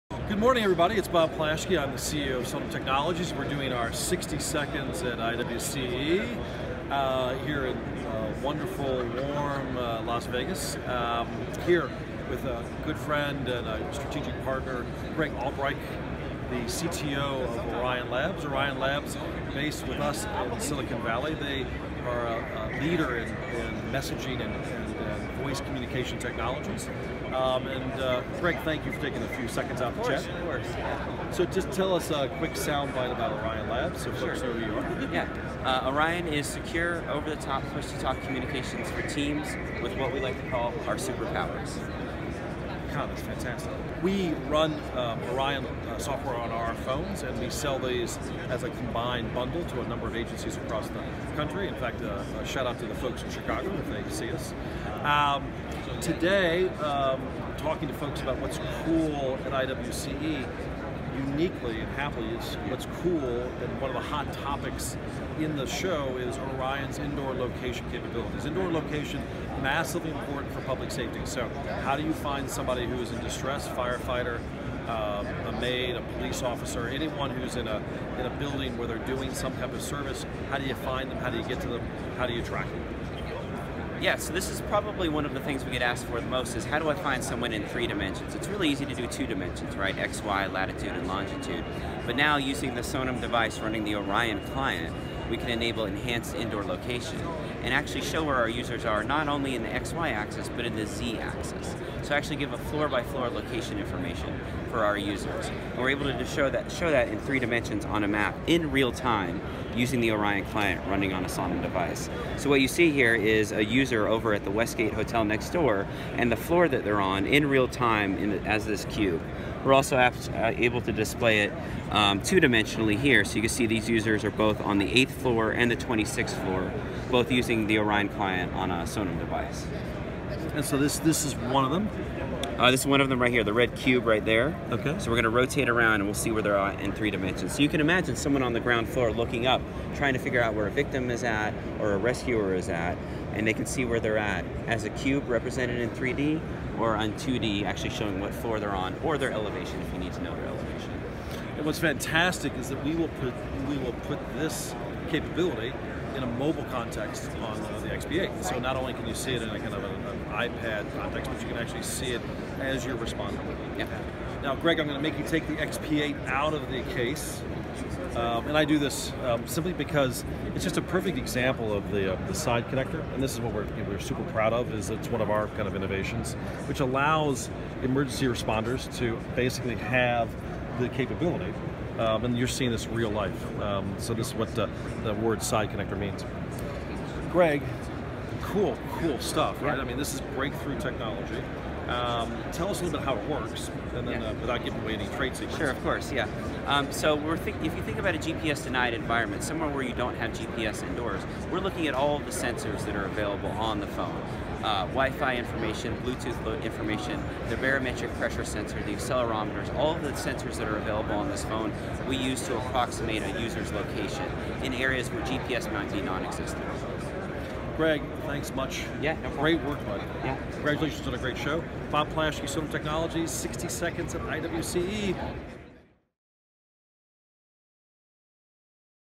OrionLabs and Sonim Technologies at IWCE 2019
When using a Sonim device & running the Orion Labs client, you can enable an enhanced indoor location solution to give users floor by floor location information in both 2D and 3D. Greg Albrecht, CTO of Orion Labs, took time to discuss this solution with our CEO Bob Plaschke at IWCE.